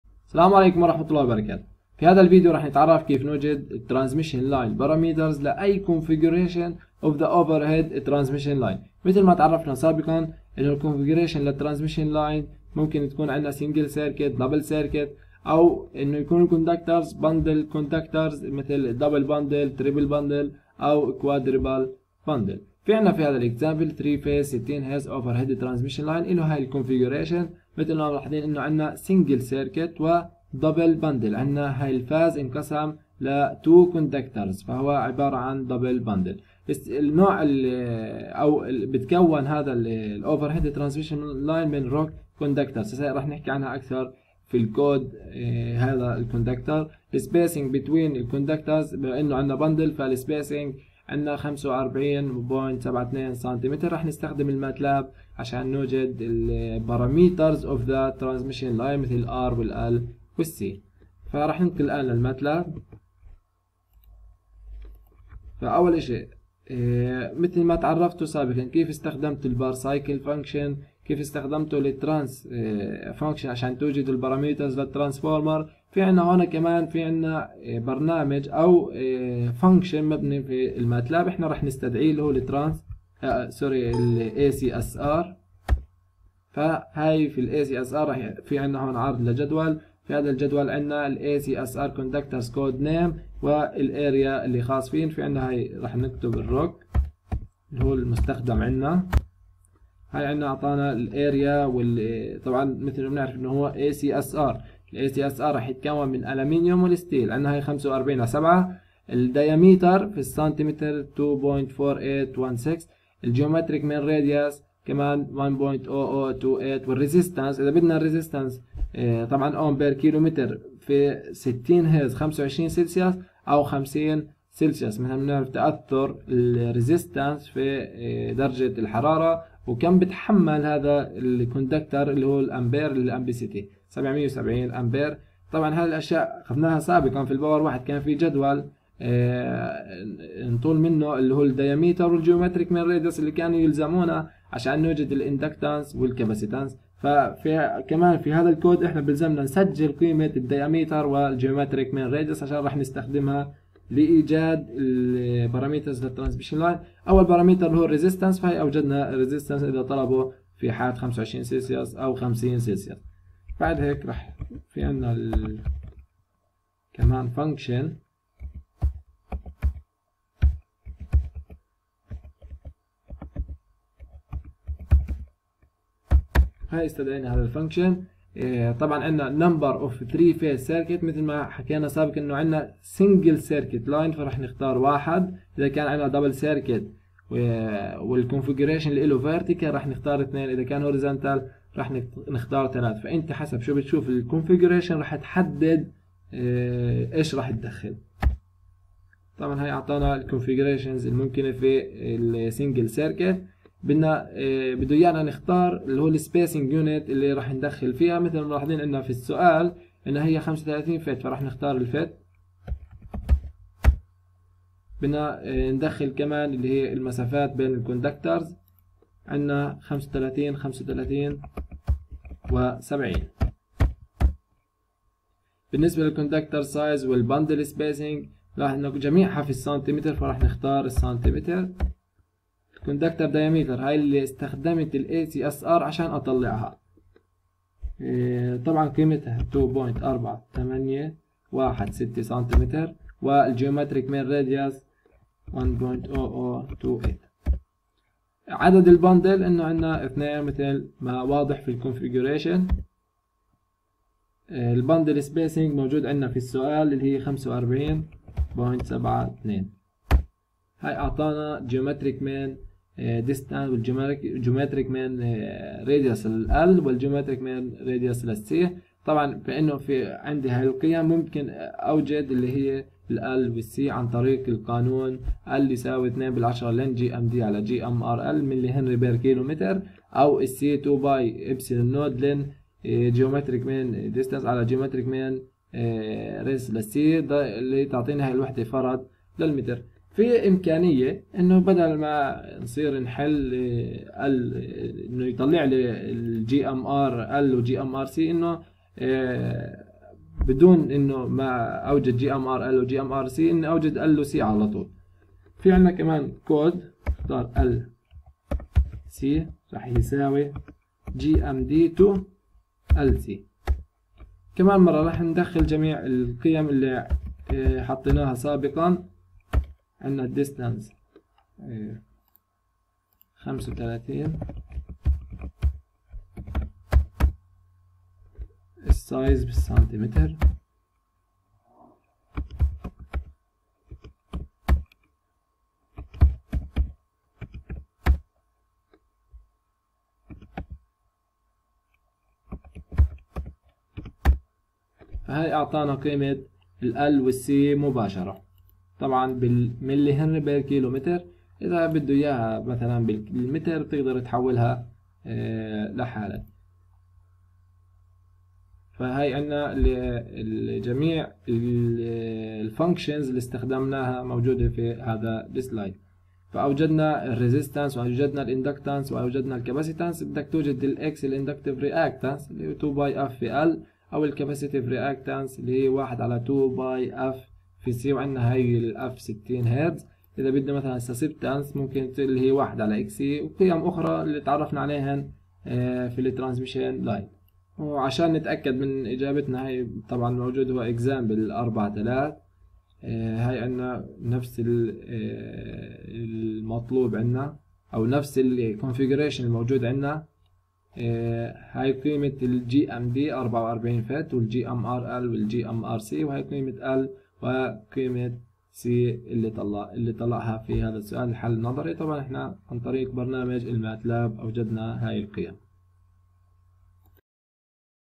Power System Planning and design || Determination of Transmission Line Parameters using MATLAB
-ACSR Program
-Parameters of TL
Supervisor: Prof. Samer Alsadi
Eng. Tareq Foqha